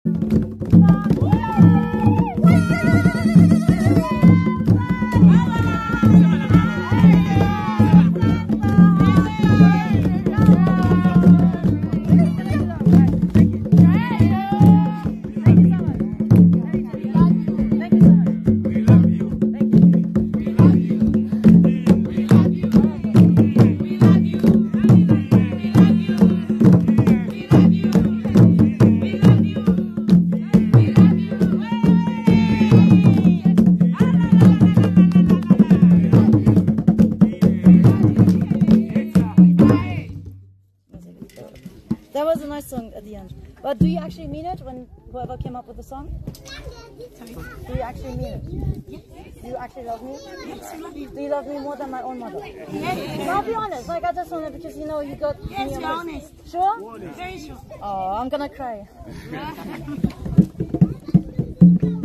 🔥 Shako Mako Gets Tribal: ZAMBIAN Drumming and Dance Takeover!
Join Shako Mako on an unforgettable journey as she immerses herself in the rich rhythms and vibrant energy of traditional African drumming and dance. 🌍 Witness her infectious enthusiasm as she learns from master drummers and dancers, showcasing her natural talent and passion for cultural exchange. ✨ Get ready to be swept away by the hypnotic beats and mesmerizing movements as Shako Mako takes you on a tribal adventure like no other! 💃